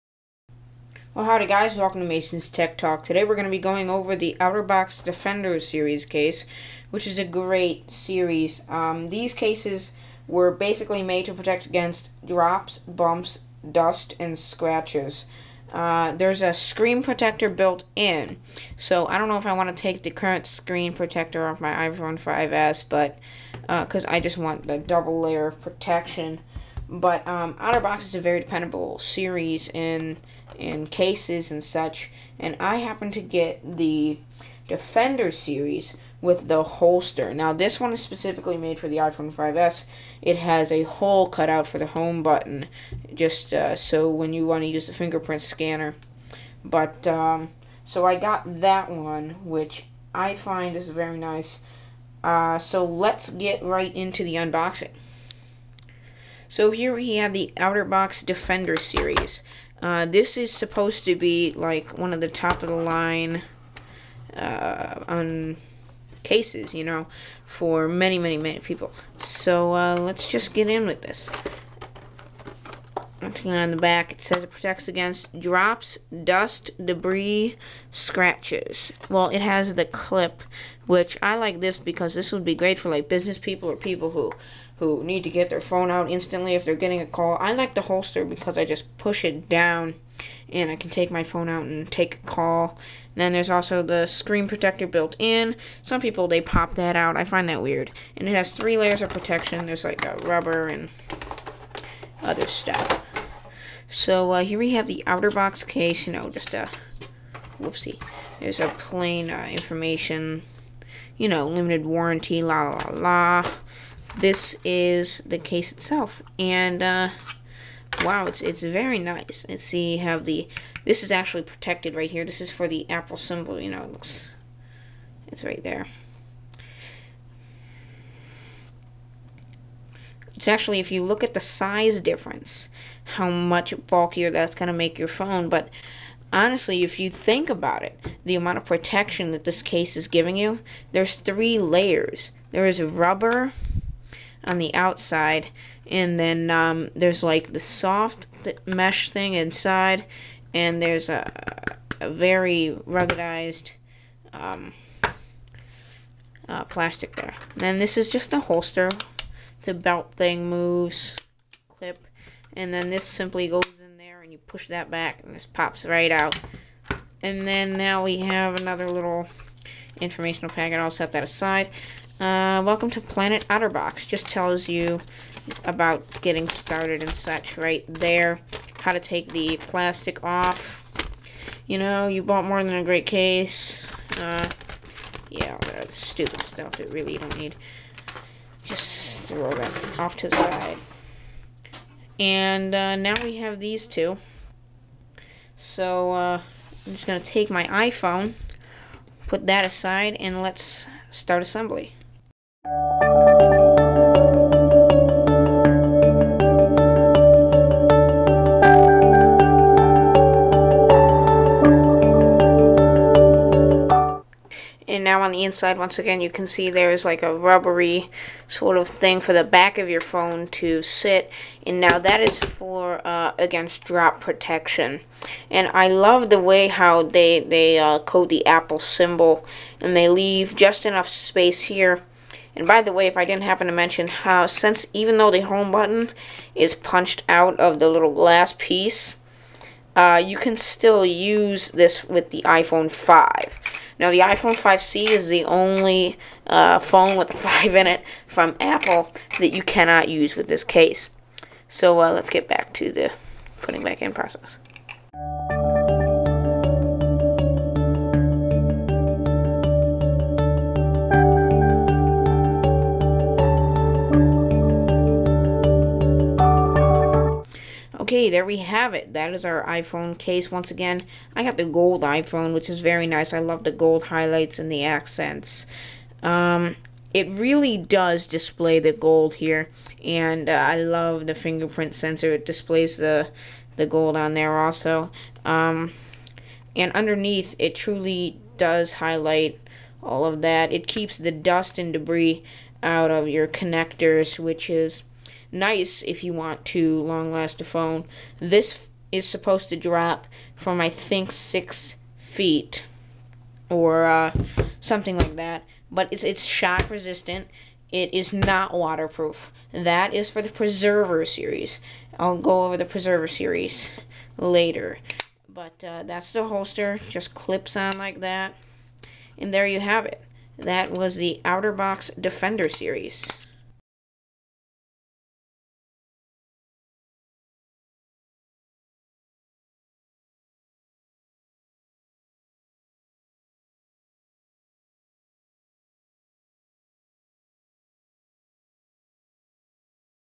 Otter Box Case Unboxing
I go over the Pro's and Cons of the Otter Box Defender Series (R) case. Thsi multi-Layerd case has a lot to talk about in this video i will show you how to put it on and how it looks on my iPhone 5S. Enjoy!